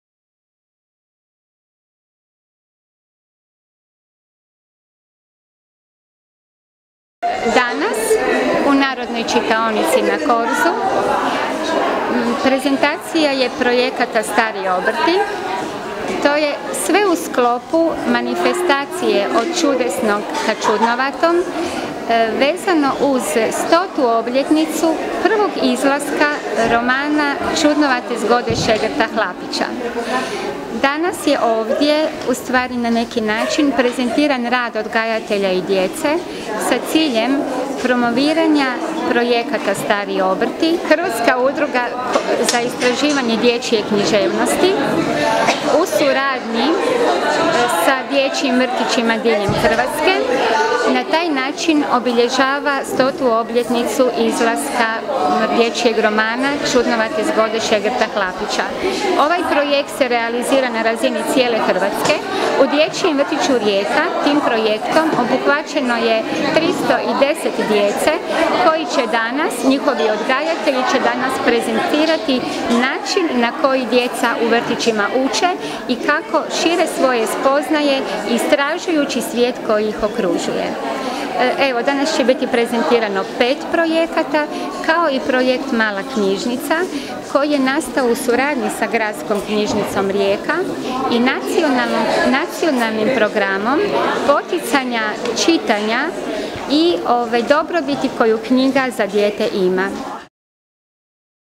MojaRijeka.hr- U Narodnoj čitaonici predstavljen projekt Stari obrti
Povodom 100. obljetnice objavljivanja romana Čudnovate zgode šegrta Hlapića Dječji vrtić Rijeka 6. lipnja, u Narodnoj čitaonici, predstavio je projekt Stari obrti.